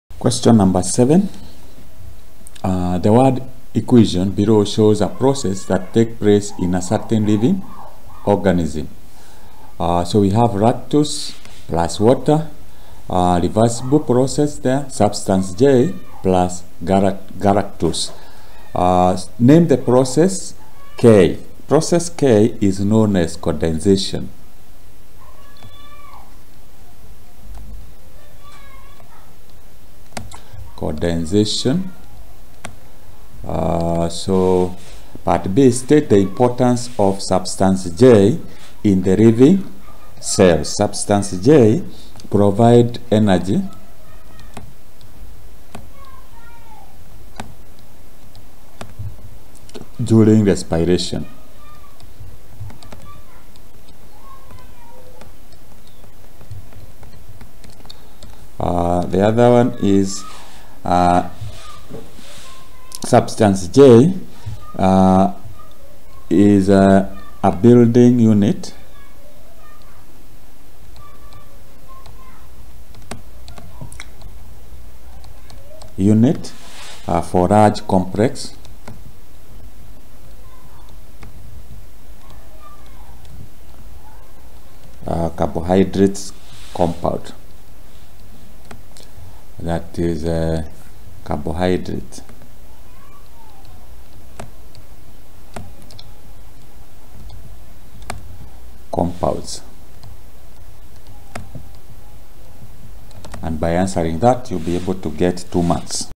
2021 KCSE BIOLOGY PAPER 1 QUESTION 7
This video explains how to arrive at the correct answer for Question 7 in KNEC's 2021 KCSE Biology Paper 1 Exam.